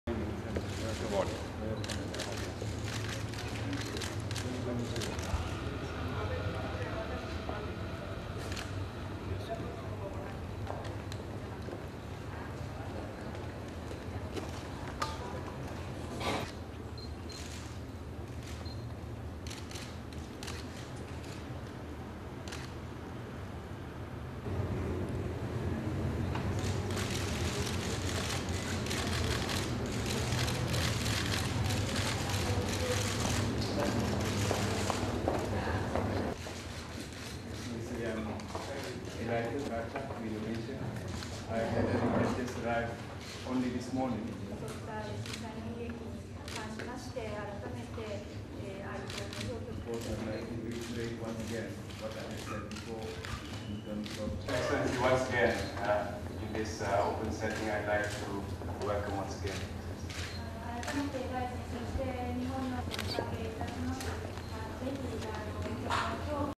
ASEAN and Japan discuss crisis management
1. Japanese Foreign Minister Takeaki Matsumoto greeted by Indonesian Foreign Minister Marty Natalegawa, they shake hands
2. Close of hands, pull out as they walk towards desk for signing of guest book
3. Matsumoto signing guest book
4. Natalegawa looking on
5. Wide of Matsumoto standing after signing and shaking hands with his Indonesian counterpart again and leaving room
6. Various of bilateral meeting
STORYLINE
Japanese and Indonesian Foreign Ministers held a bilateral meeting in the Indonesia capital Jakarta on Saturday, ahead of the meeting between ASEAN (Association of Southeast Asian Nations) and Japan.
Japanese Foreign Minister Takeaki Matsumoto and Indonesian Foreign Minister Marty Natalegawa discussed various issues relating to the earthquake and tsunami disaster which struck Japan almost one month ago.
Indonesia, which holds the chair of ASEAN, has taken the initiative by hosting a special meeting of ASEAN ministers in Jakarta.
Ministers of ASEAN and Japan will discuss ways and means to improve natural disaster management cooperation and assistance, including the post-disaster recovery efforts, rehabilitation and reconstruction.
The meeting also aims to strengthen cooperation in aspects such as pre-disaster prevention and mitigation, and disaster risk reduction.